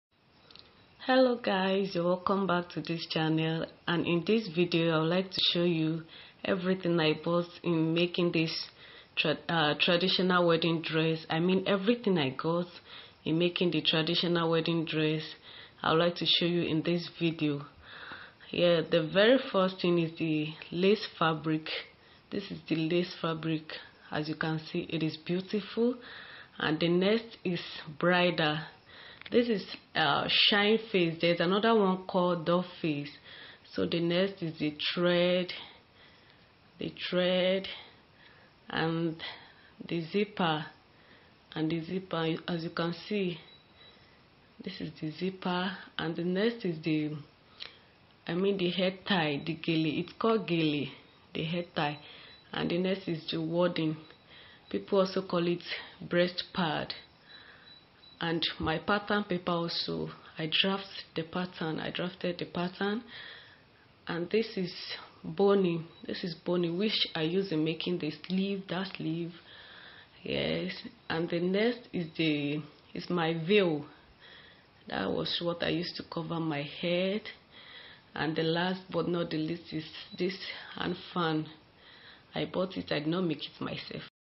Things I bought for making my traditional wedding dress 💝
This is a video showing everything I bought for making my elegant traditional wedding dress 💝